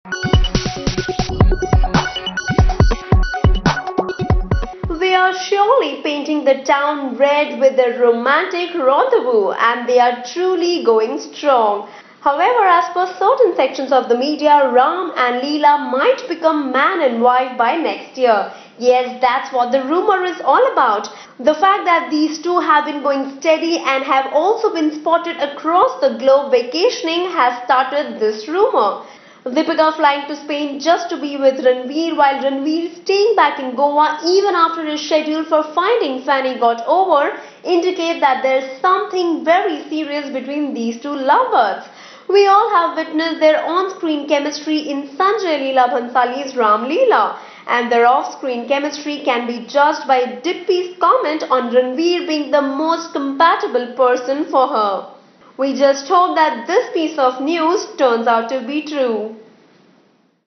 REALLY?? Ranveer Singh & Deepika Padukone's Wedding Next Year?? | Hot Bollywood News | Finding Fanny
If sources are to believed then the sizzling ‘Ram Leela’ Jodi - Ranveer Singh and Deepika Padukone - is going to tie the knot next year...!!